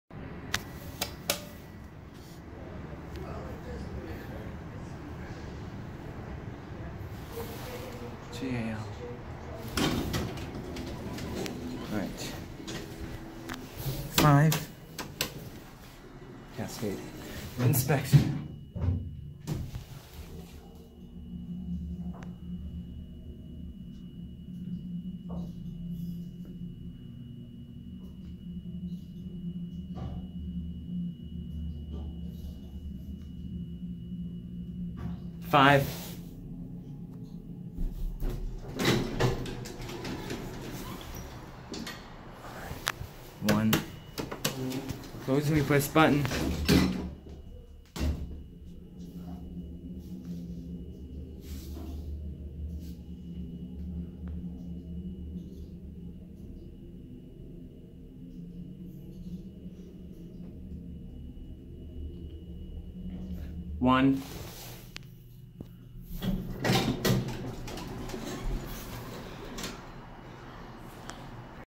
First 1994 O&W Hydraulic Elevator @ Camelot Inn & Suites, Anaheim CA
O&W stands for Oliver and Williams. Located near the entrance to Disneyland Park.

Taken 26 December 2019

Installed 1994